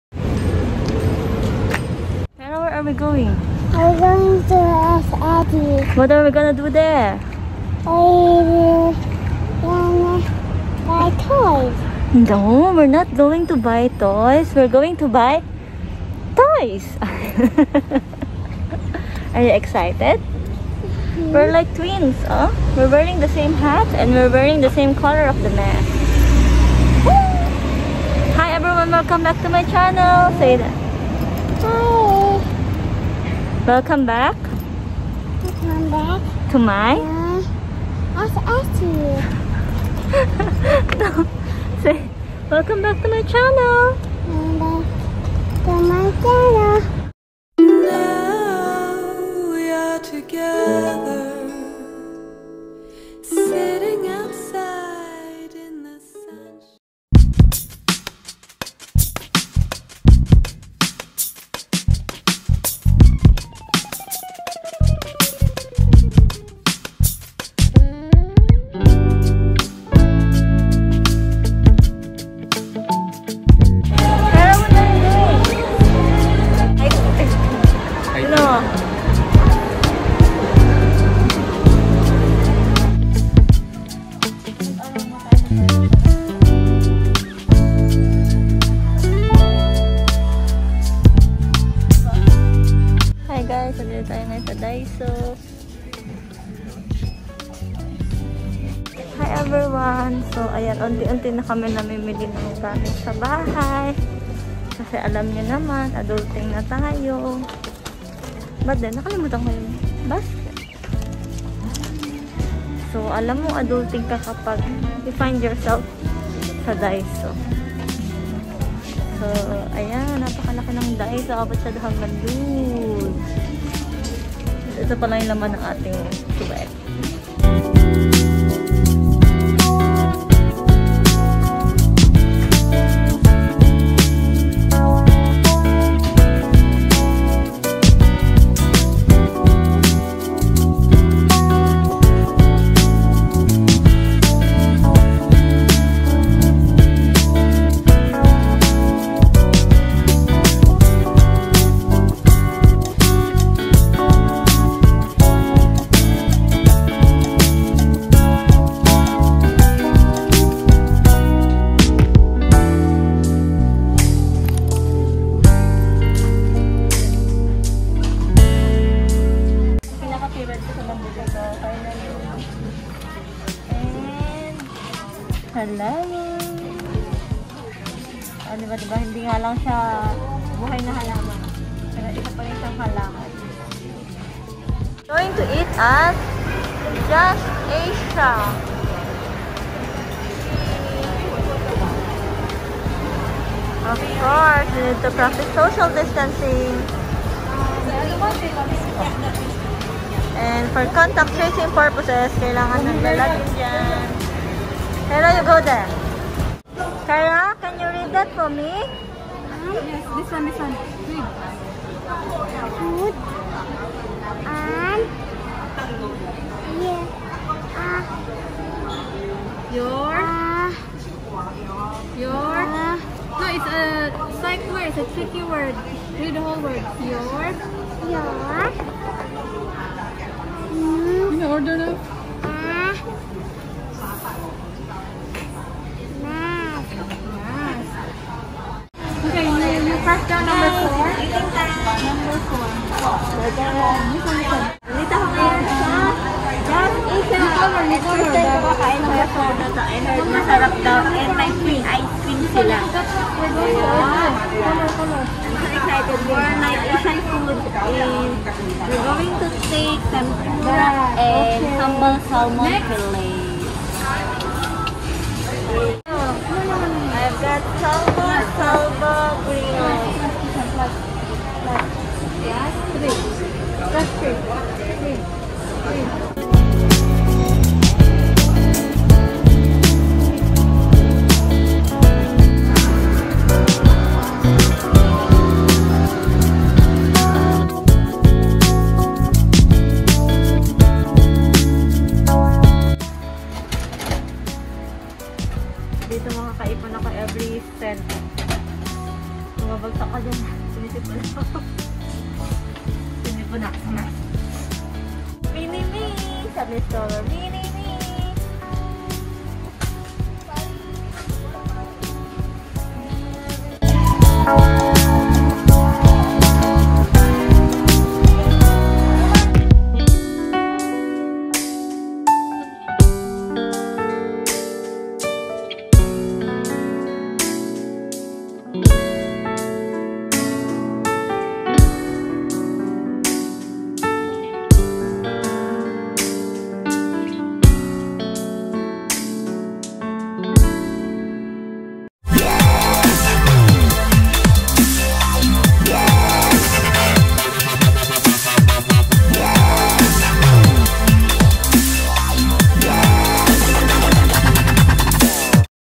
Shopping with Housemates in Singapore. / Buhay OFW
We bought some things at Sembawang Shopping Center.